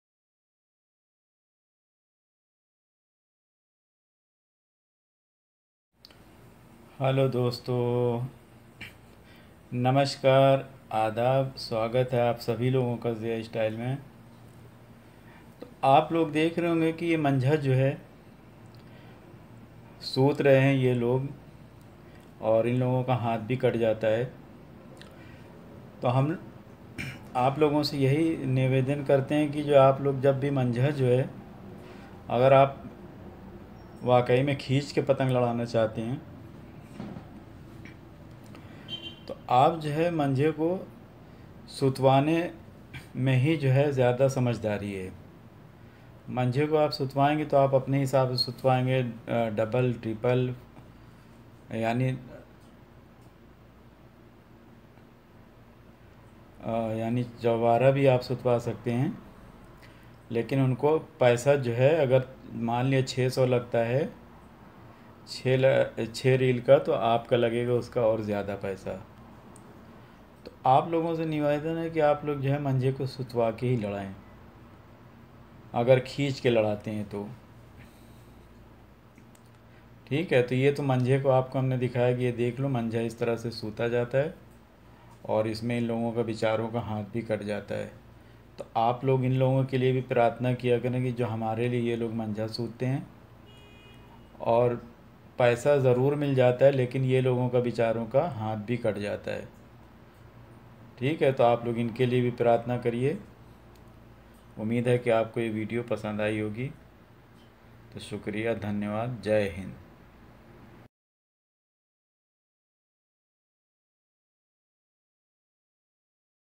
मंझा सुतवाना भी चाहिए /manjha sutavaana bhee chaahie/hindi urdu
मंझा सुतवाना भी चाहिए/manjha sutavaana bhee chaahie//Manjah Sutvana should also/ये विडियो में किस तरह का मांझा सुतवाये हमने समझने की कोशिश की है/ye video men kis tarha ka manjha sutwaye hamne samjhane ki koshis ki hai/We have tried to understand the kind of mood in this video.